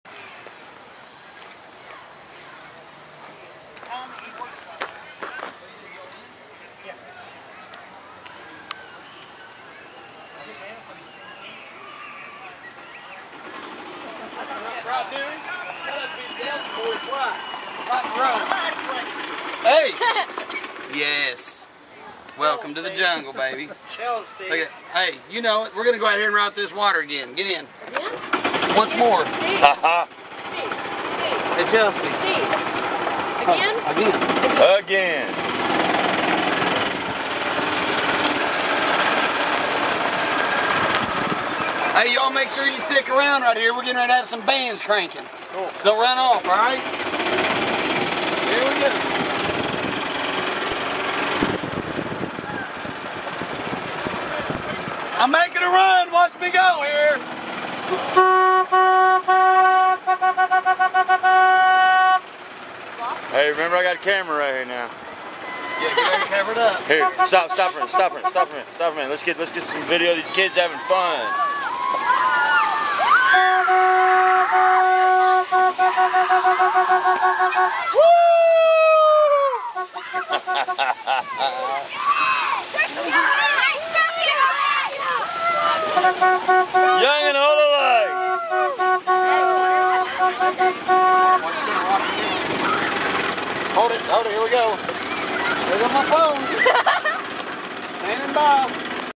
The Greenup Fire Dept Washes Us Out
The fire dept showed up to cool things down.